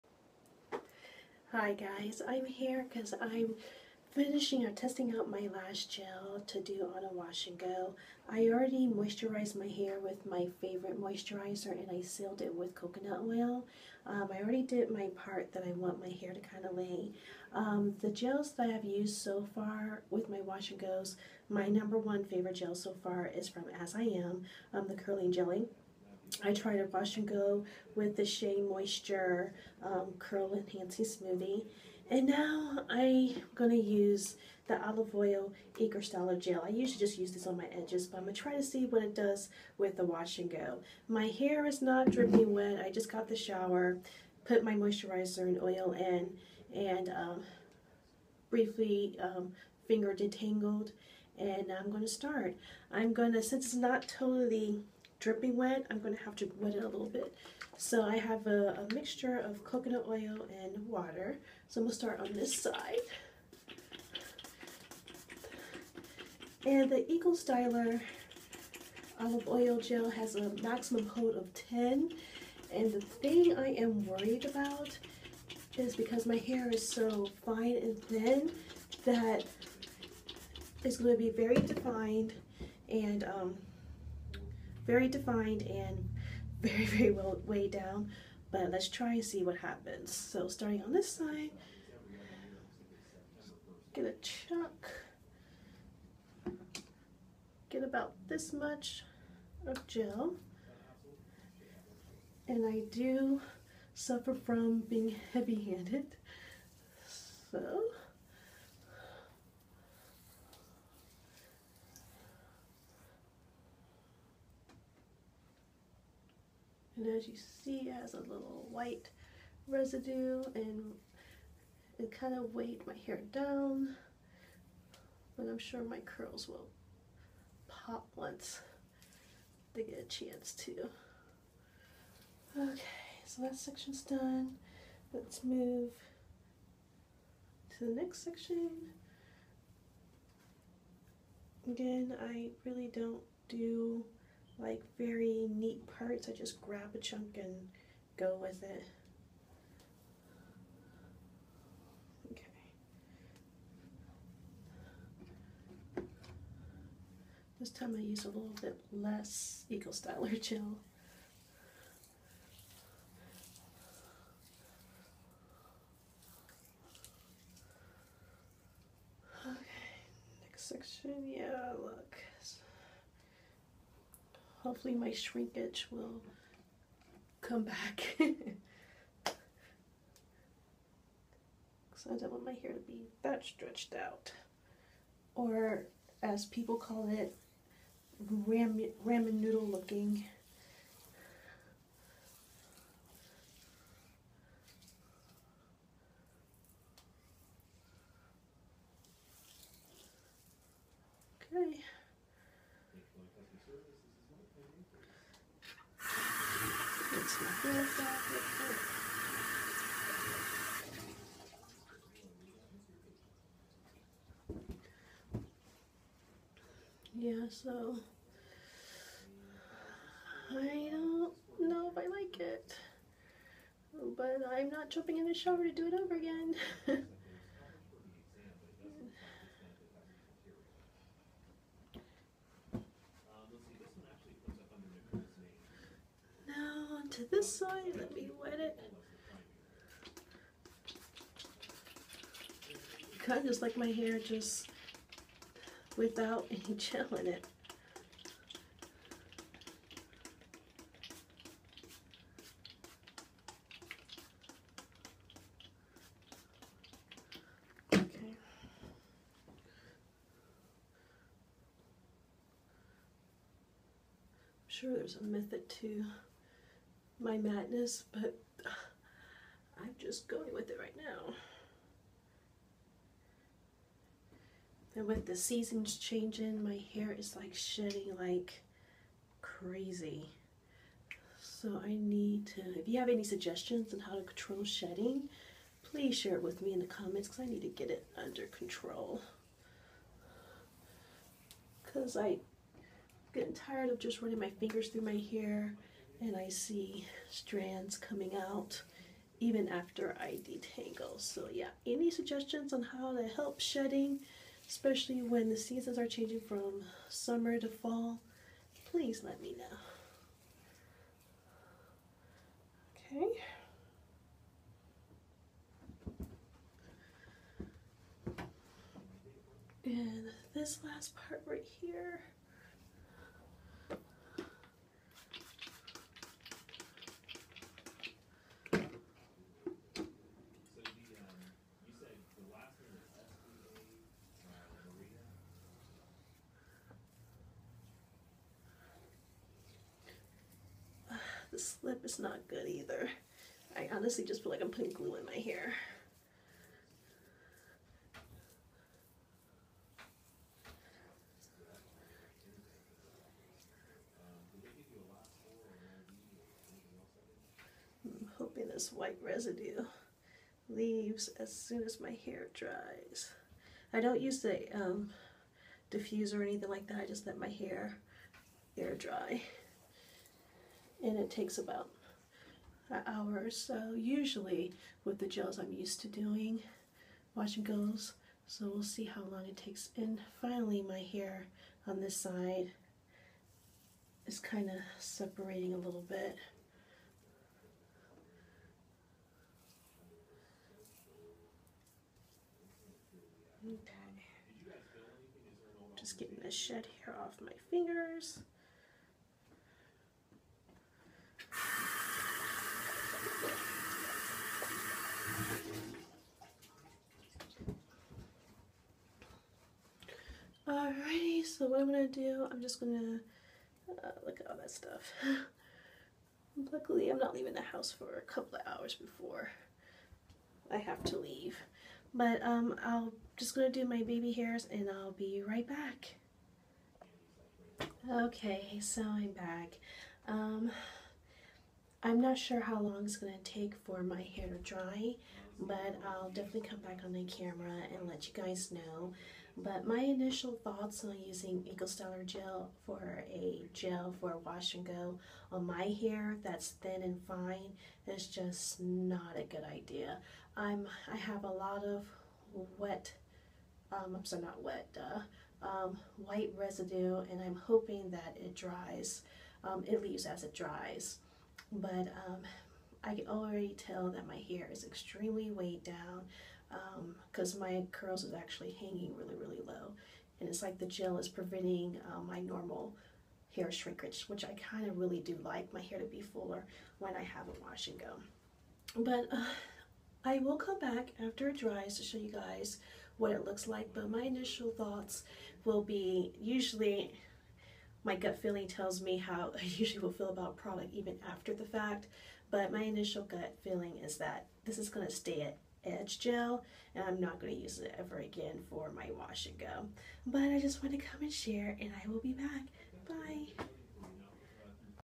Last trial of a perfect gel for wash and go on fine hair: Eco styler olive oil gel
The last gel I am trying to get the best wash and go results. The gel featured in this video: Eco Styler Olive Oil Gel. Watch and find out my thoughts